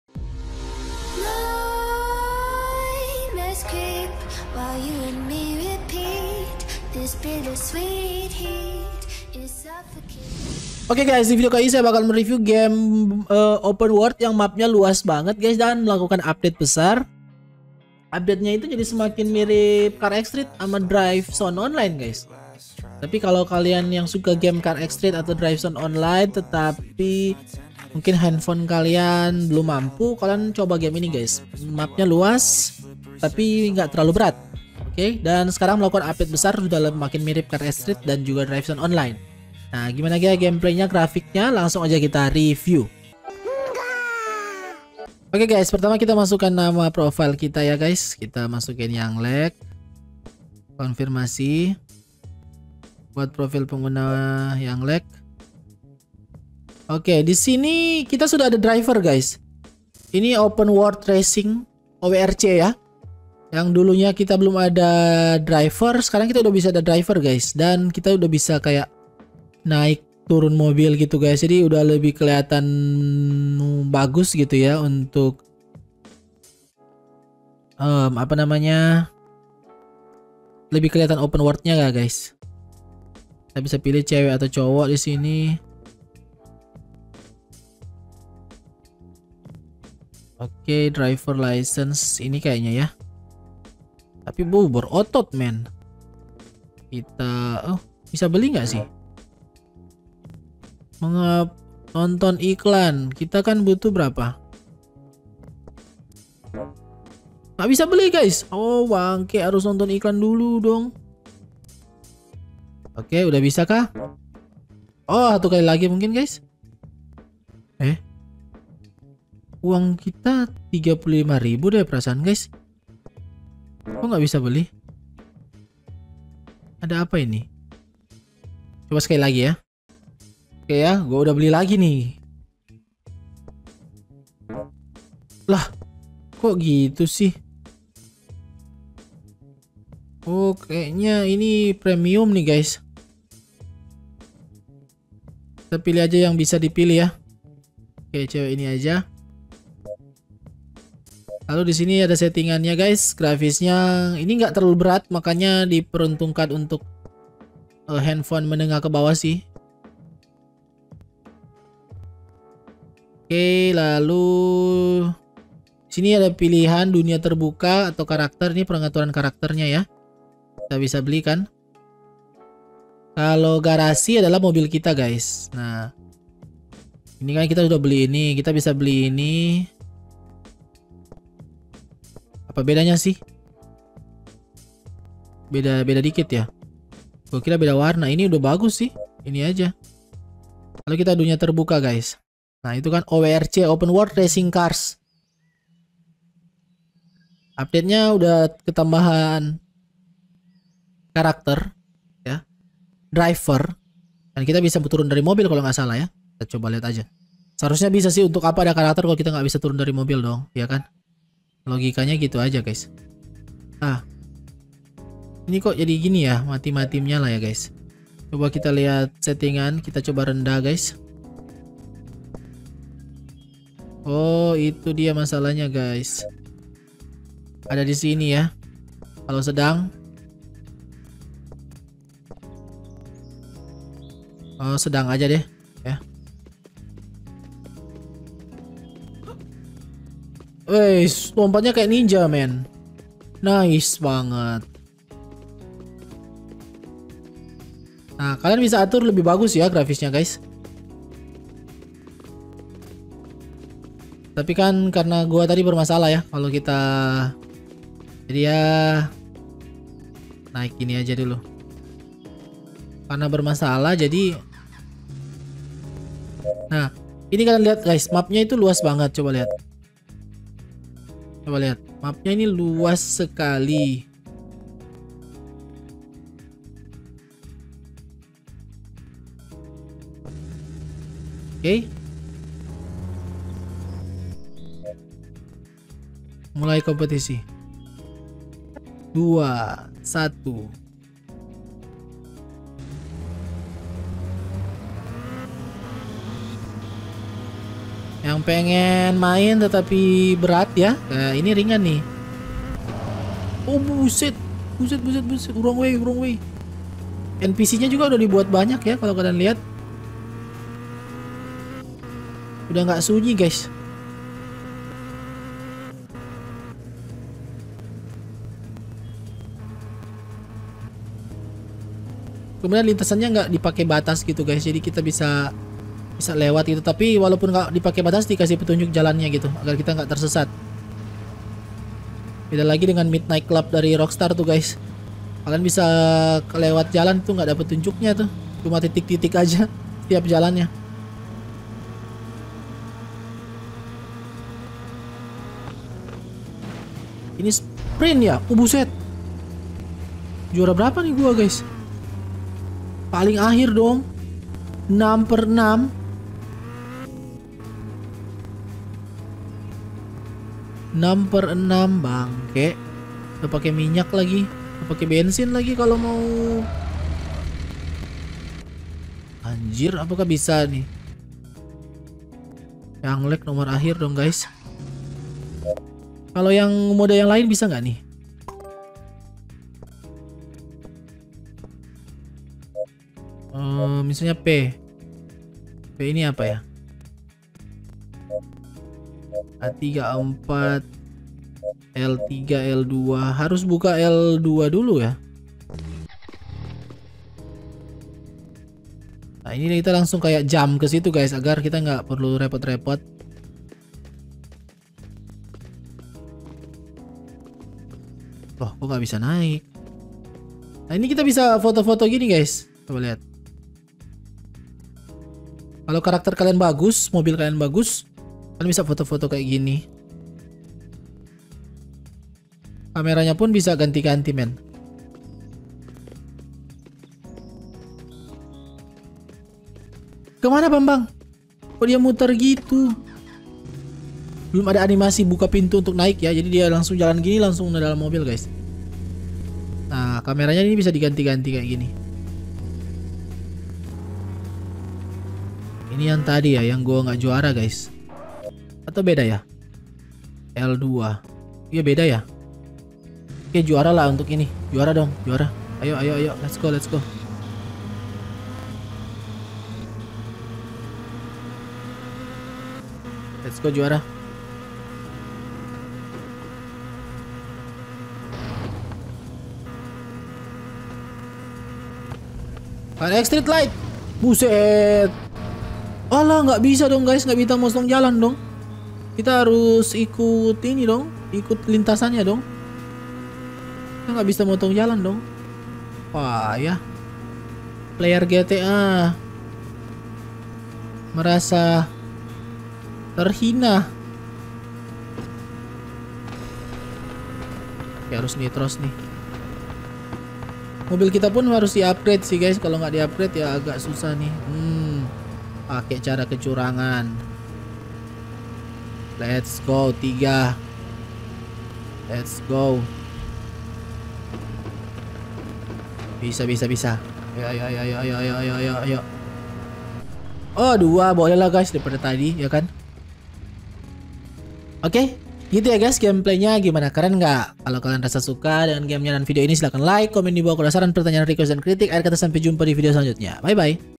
BIG UPDATE ! Jadi Car X Stree LITE Nih ! Game Open World Android TERBAIK Map LUAS BANGET !
Game Open World Terbaik Android yang melakukan update besar guys, updatenya jadi mirip Carx Street tapi ukurannya ringan loh^^